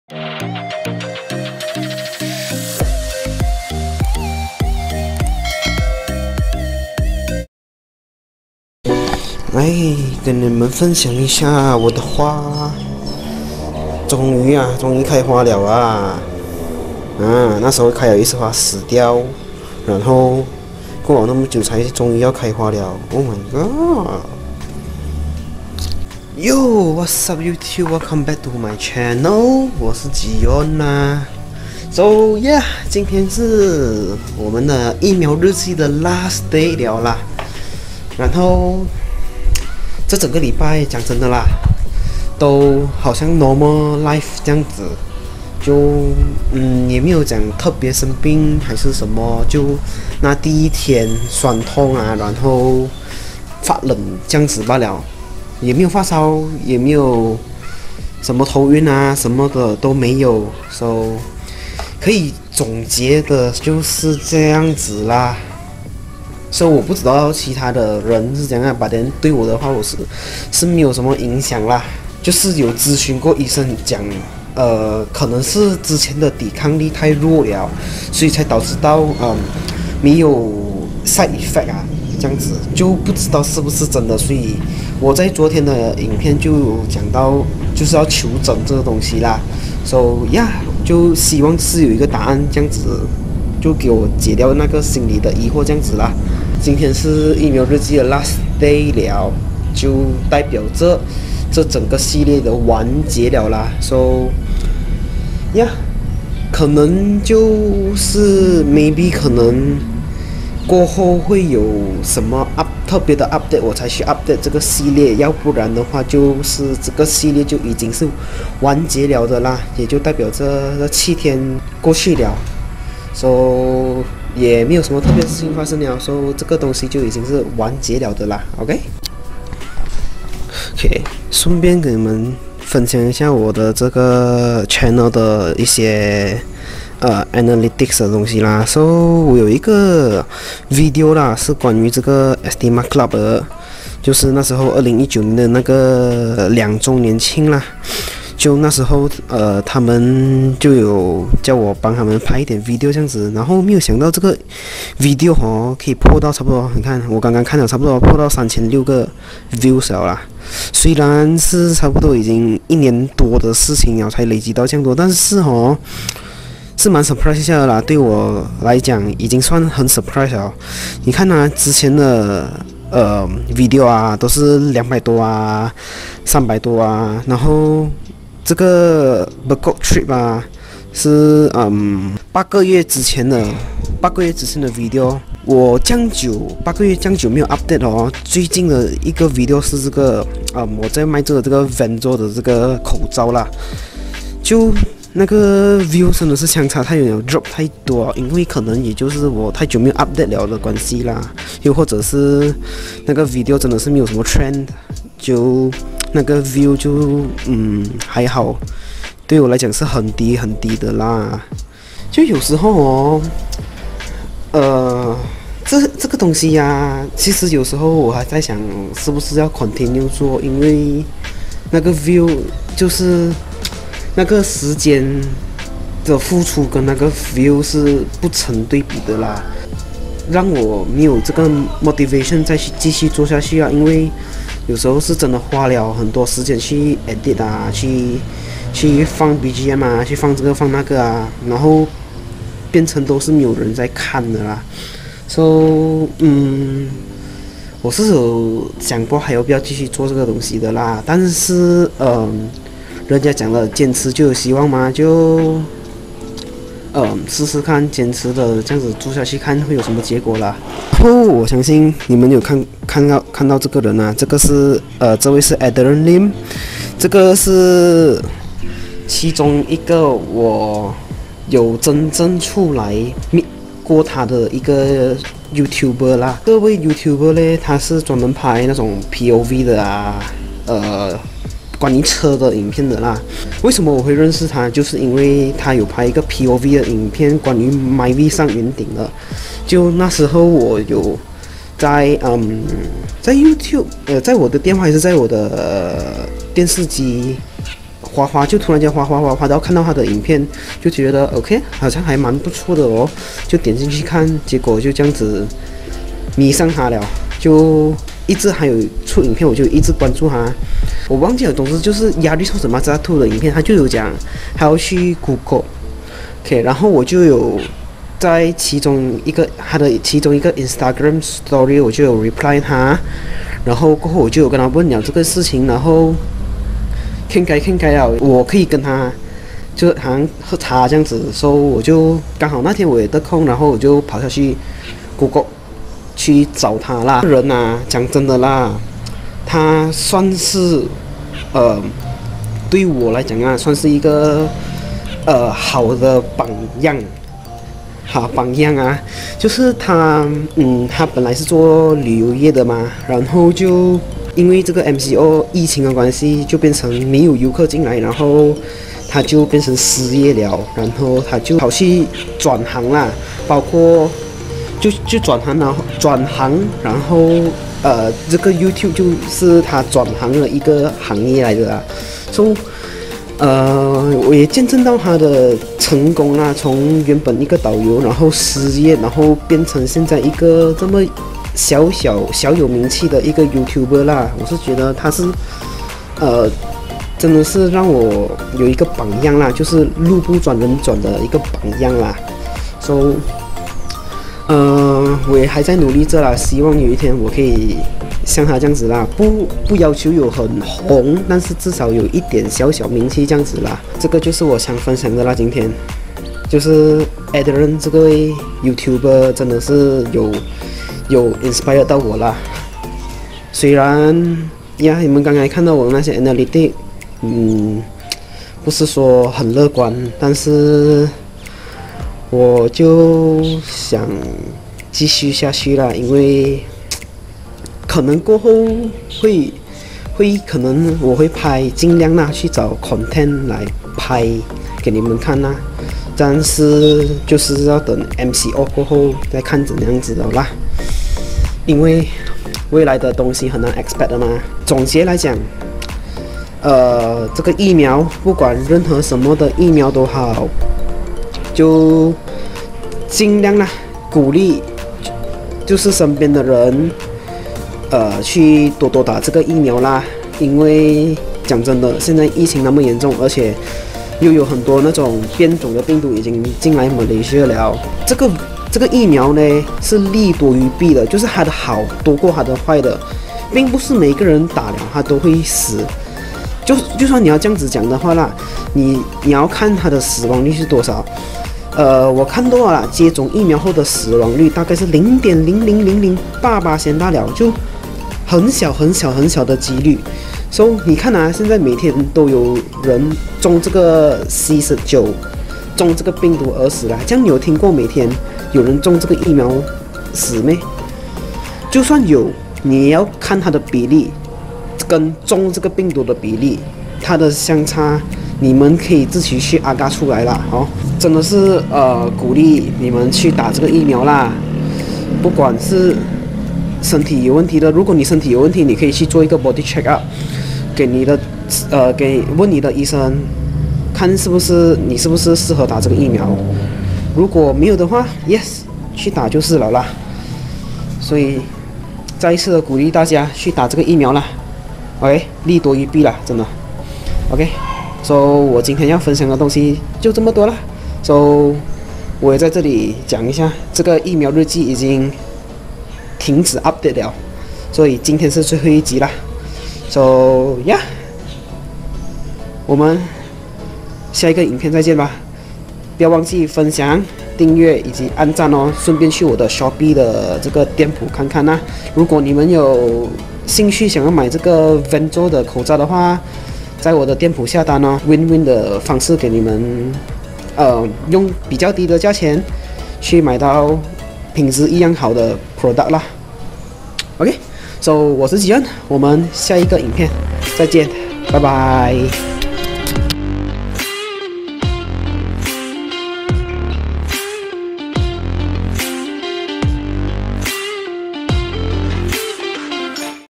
【疫苗日记 S1 Day 7】7天里的总结 & 影片的启发
疫苗日记是为了记录打了疫苗后身体所出现的状况
疫苗日记将会连续7天更新 所以请大家多多支持~

别忘记follow我的shopee网店 可以的话下单以示支持哦 谢谢！